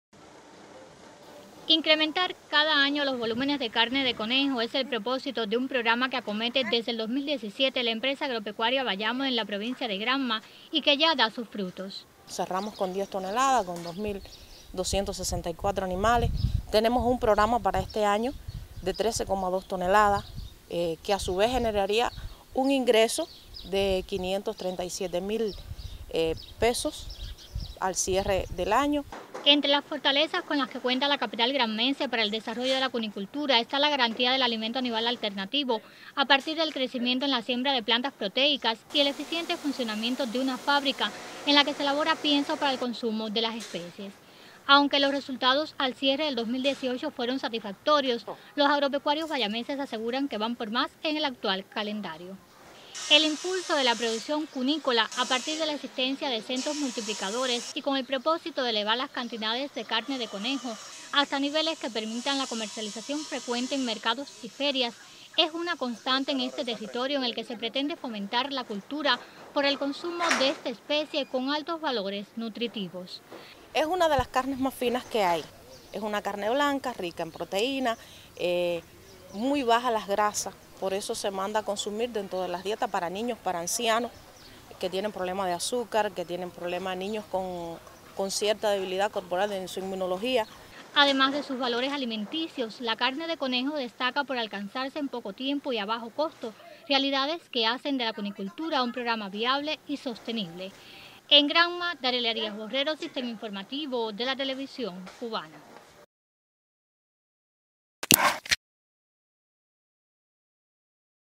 Empeños para fomentar la cultura cunícola en la alimentación de los bayameses
En el 2018 la Empresa Agropecuaria Bayamo cumplió sus compromisos correspondientes al programa cunícola. Para el actual calendario la empresa tiene el encargo de incrementar esos volúmenes para alcanzar niveles que permitan la comercialización frecuente en mercados, ferias y otros espacios.